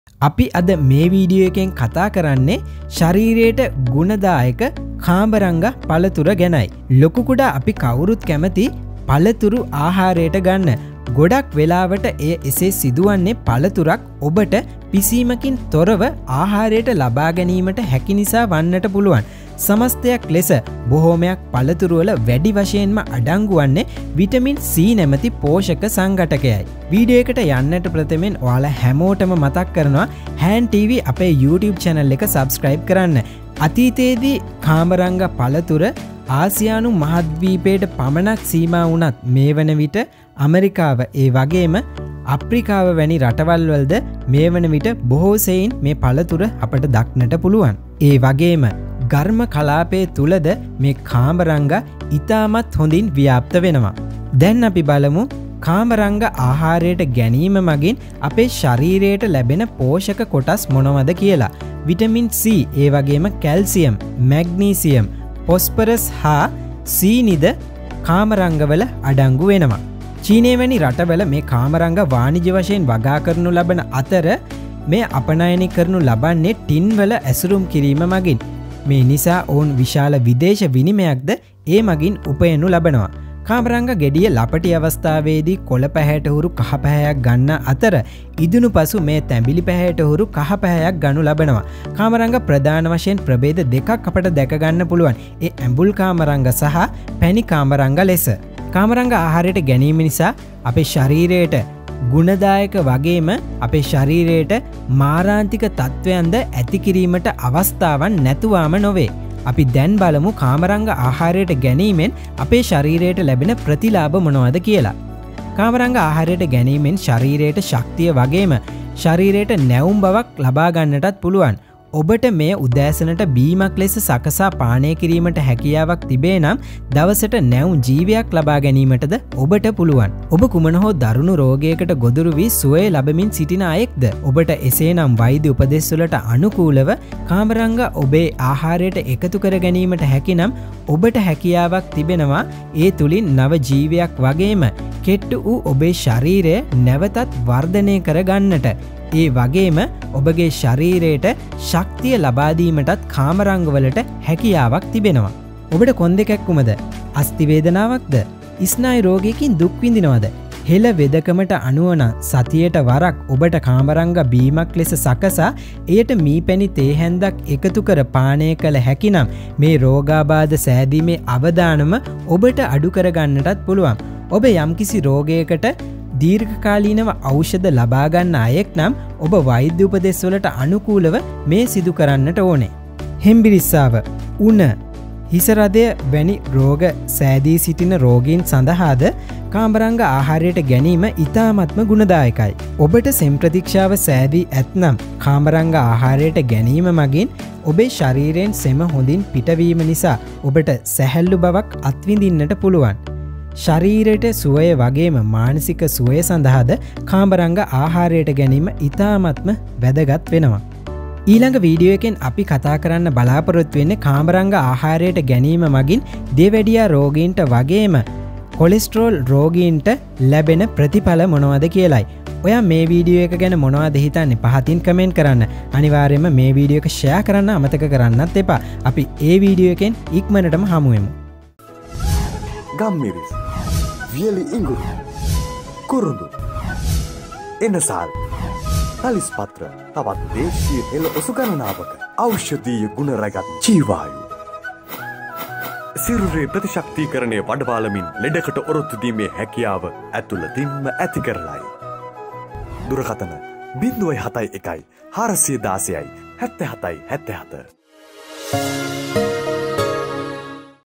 Star Fruit in Sinhala | Healthy Benefits | Hand TV
Carambola, also known as star fruit or starfruit, is the fruit of Averrhoa carambola, a species of tree native to tropical Southeast Asia.The fruit is commonly consumed in parts of Brazil, Southeast Asia, South Asia, the South Pacific, Micronesia, parts of East Asia, the United States, and the Caribbean. The tree is cultivated throughout tropical areas of the world.